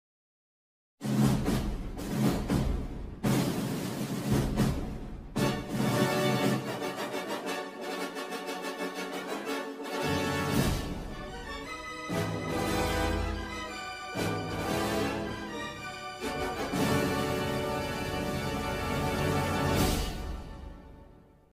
1996 20th century fox home entertainment in My Plotagon major
How to make My Plotagon major

Instructions

Gradient map: Black, Purple & White

Audio: Yellow bomb with farts

Logo: Plotagon